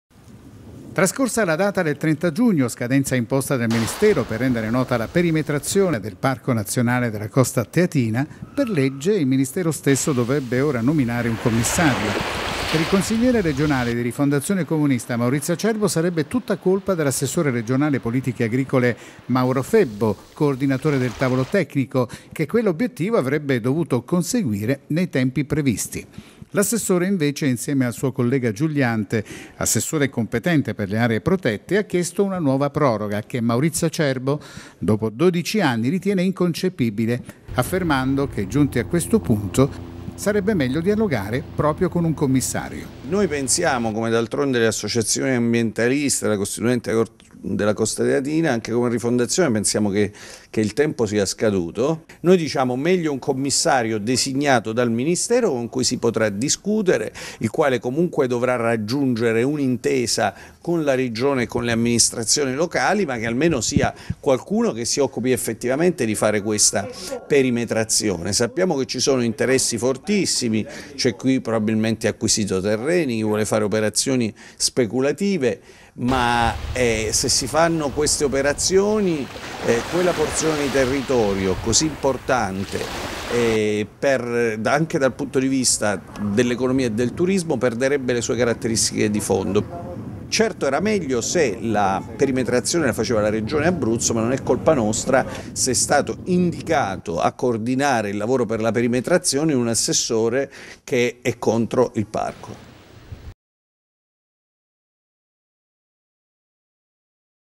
Parco, Acerbo vuole il commissariamento
Lanciano, servizio Tgmax 1 luglio 2013. Parco della costa: tempo scaduto per la perimetrazione, il consigliere regionale Maurizio Acerbo (Rifondazione comunista) chiede il commissariamento "Troppi interessi speculativi".